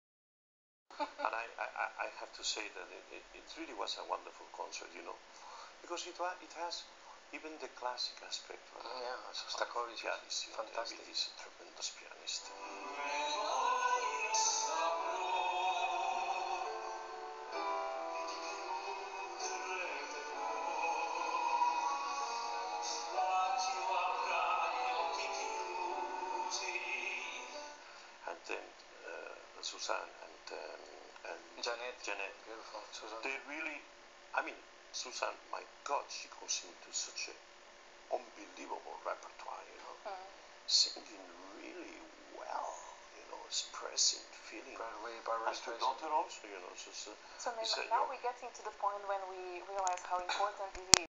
Video Mr.Placido Domingo about Zuzana Martinsen and Con Amore concert
Placido Domingo  and Placido Domigo Jr. about Concert Con Amore   and about singer Zuzana Martinsen and Jeanette Susann